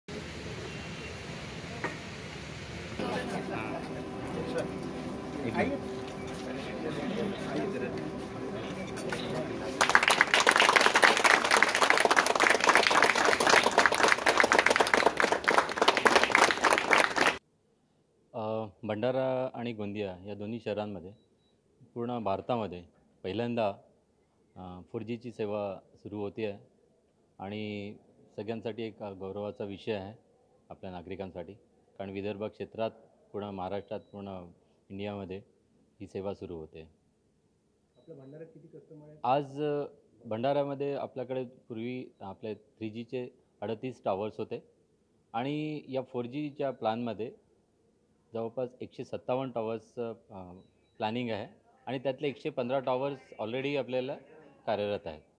बीएसएनएलची ४जी सेवा देशात सुरू, भंडारा जिल्ह्याला मिळाला पहिला मान
भंडारा - सर्वसामान्य लोकांच्या सर्वात जवळच्या समजल्या जाणाऱ्या बीएसएनएलने आपल्या मोबाईल सेवेमध्ये प्रगती करत आपली पहिली 4जी सेवा आज भंडारा जिल्ह्यात सुरू केली आहे. यावेळी खासदार मधुकर कुकडे, सुनील कुमार, एस. के. मिश्रा, आणि इतर मान्यवरांच्या हस्ते या सेवेची विधिवत सुरुवात करण्यात आलीा आहे. महाराष्ट्रात बीएसएनएलने सर्वात प्रथम भंडारा आणि गोंदिया जिल्ह्यात ही 4जी सेवा सुरू असून, येत्या काळात संपूर्ण महाराष्ट्रात ही सेवा सुरू केली जाणार आहे.